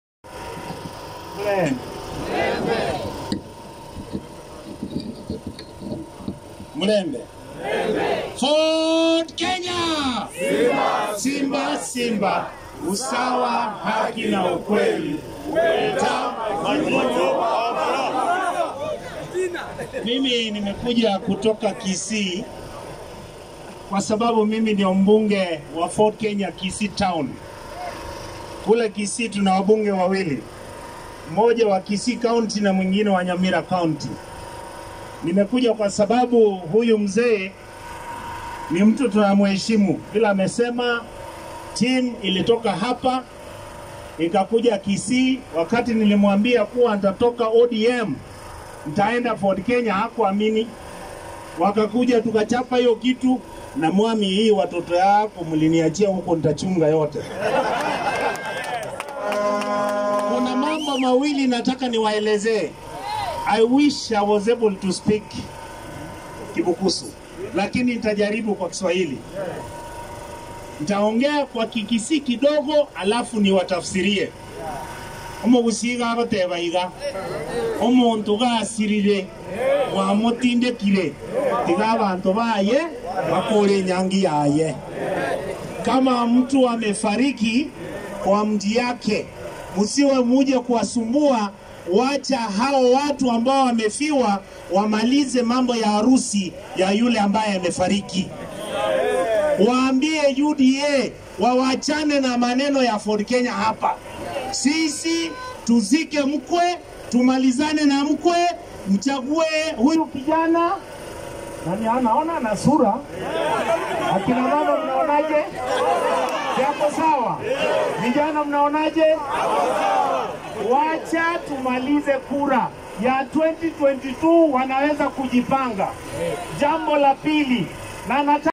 Ford Kenya deputy leader Richard Onyonka campaigns for FK aspirant in Kabuchai by-poll Majimbo Kalas
Ford Kenya party deputy leader Richard Onyonka campaigns for FK aspirant in Kabuchai by-election, Majimbo Kalas